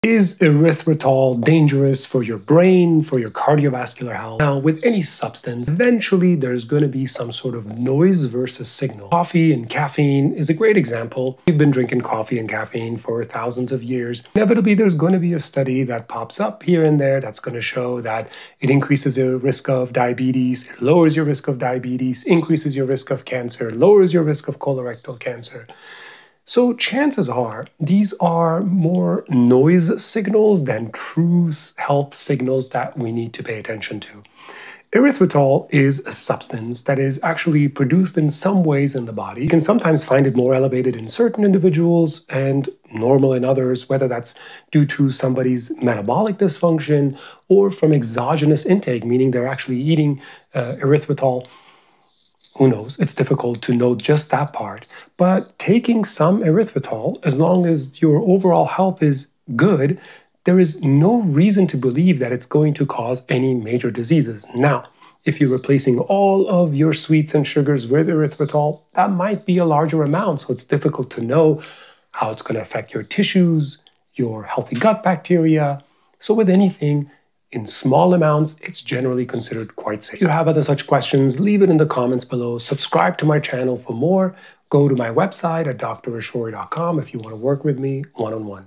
Is Erythritol Safe for Your Health? Doctor Ashori Explains
Erythritol is a common sugar substitute that often makes headlines. Some studies suggest possible risks, others show no harm. So what does the science actually say?

The truth is that erythritol is produced naturally in small amounts by the body, and when consumed in moderation, it is considered safe for most people. Concerns usually arise when it is used in very large amounts, especially if it replaces all other sources of sweetness.

The key is balance. Small amounts of erythritol are unlikely to cause major issues, but it is always better to focus on whole foods, natural eating patterns, and overall metabolic health.

For more healthy aging content, visit drashori.com.